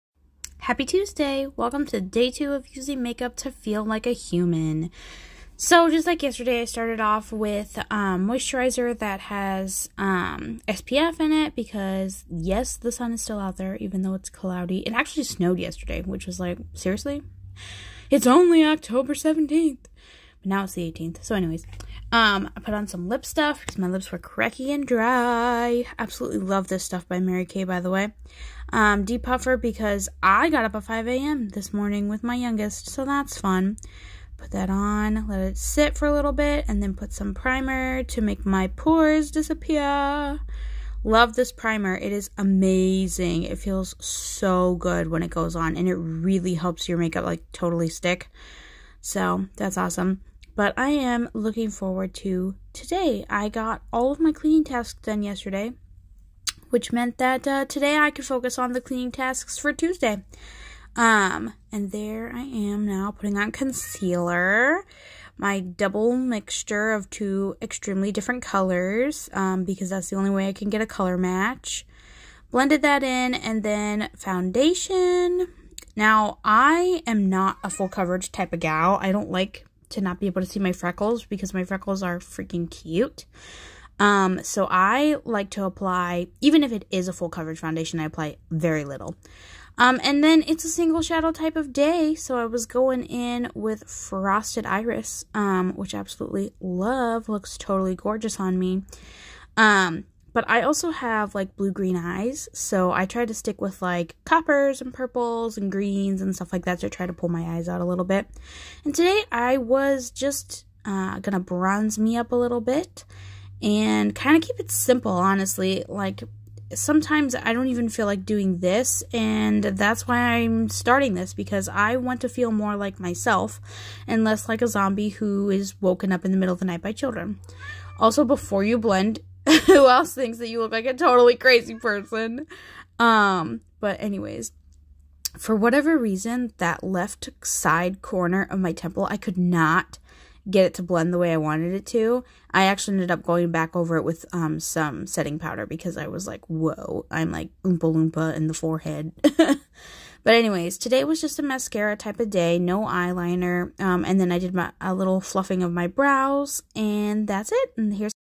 Episode 2 of using makeup to feel like a human!
this is day two of using make up to feel like myself again. I hope you all enjoy watching. almost all the products I'm using are Mary Kay.